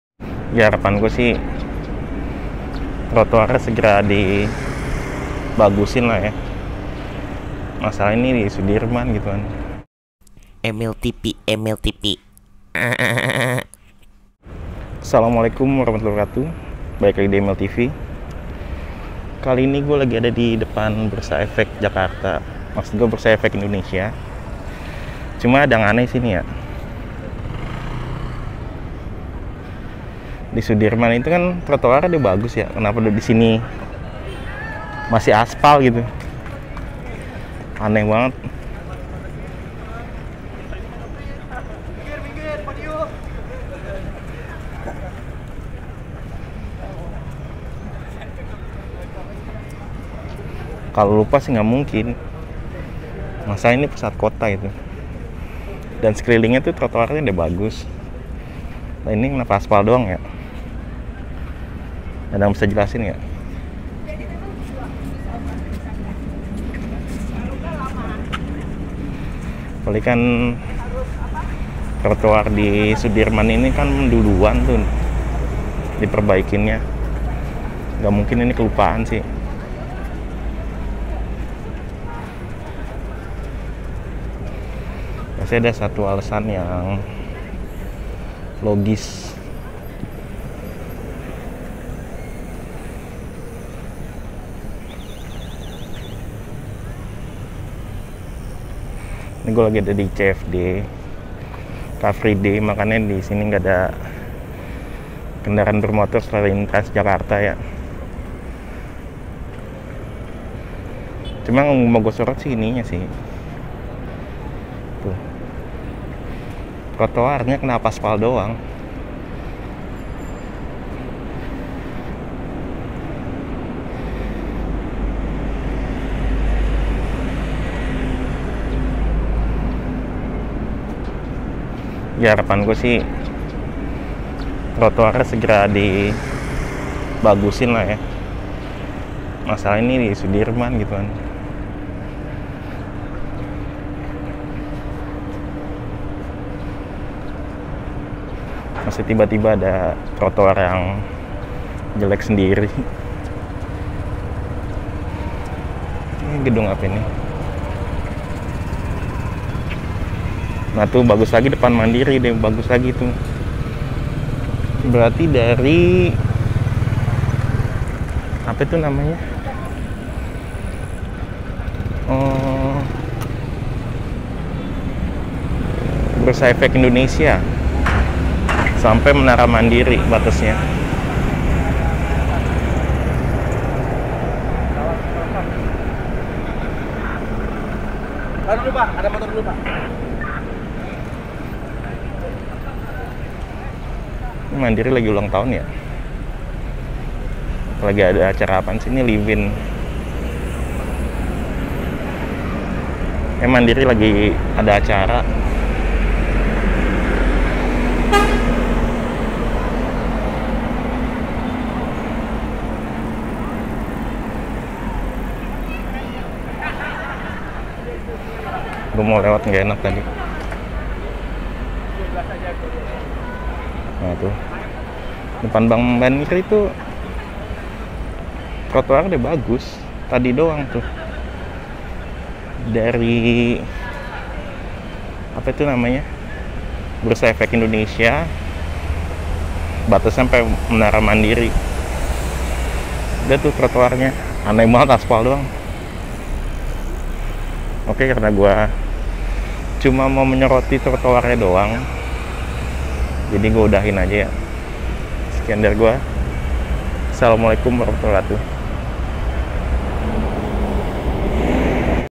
Ternyata di Jl. Jenderal Sudirman masih ada loh trotoar yang jelek. Kok bisa ya?
Revitalisasi trotoar di Jakarta sudah sangat pesat. Terutama di jalan-jalan besar, salah satunya Jalan Jenderal Sudirman. Tapi ternyata ada trotoar di Jalan Jenderal Sudirman yang belum dipercantik, tepatnya ada di depan gedung Bursa Efek Indonesia. Kenapa? Saya juga belum tahu.